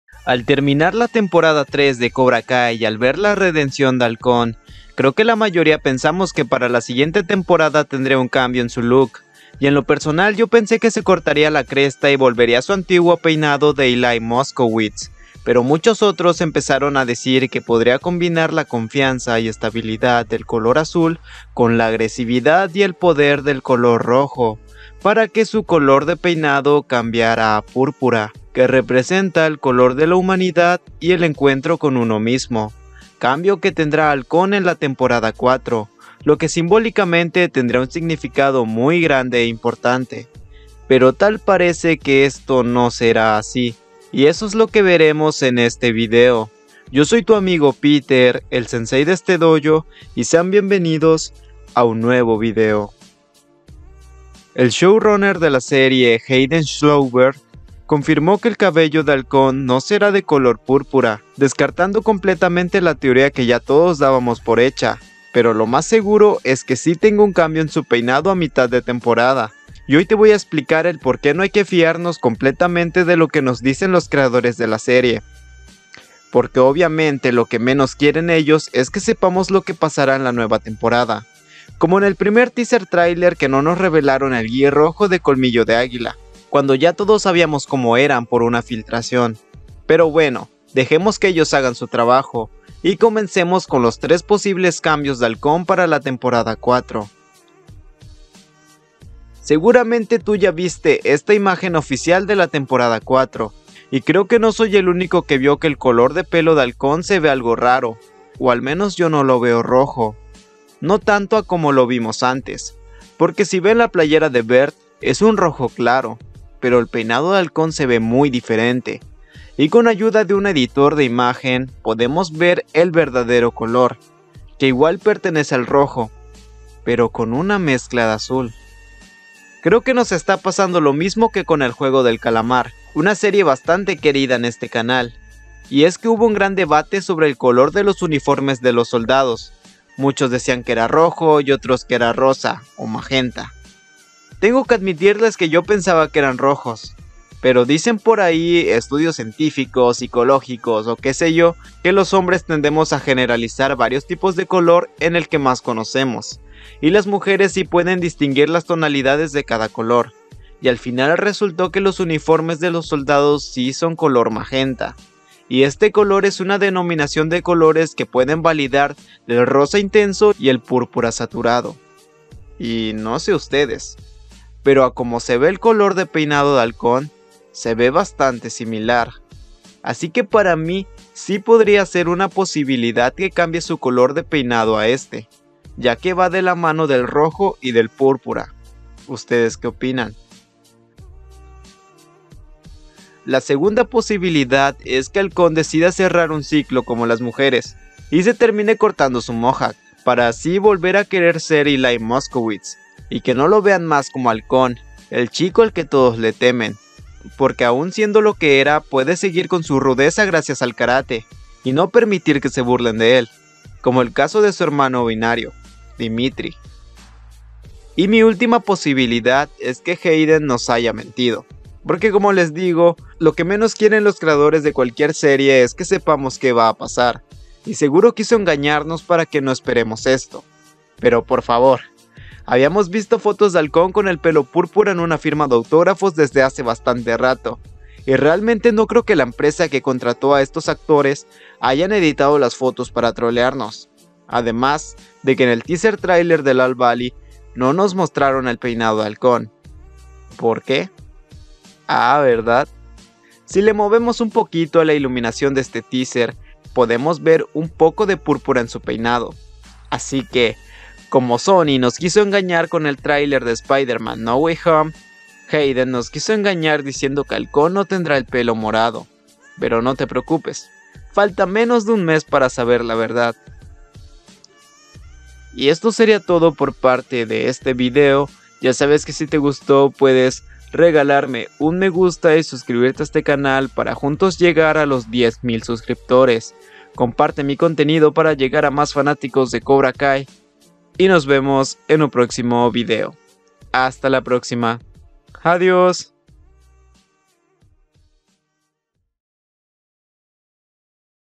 EL COLOR DEL MOHAWK DE HAWK NO SERÁ MORADO!? ¿De que color será? Filtraciones Cobra Kai Temporada 4
Si el color del Mohawk de Hawk no es purpura, entonces de que color será?

¡MUCHAS GRACIAS!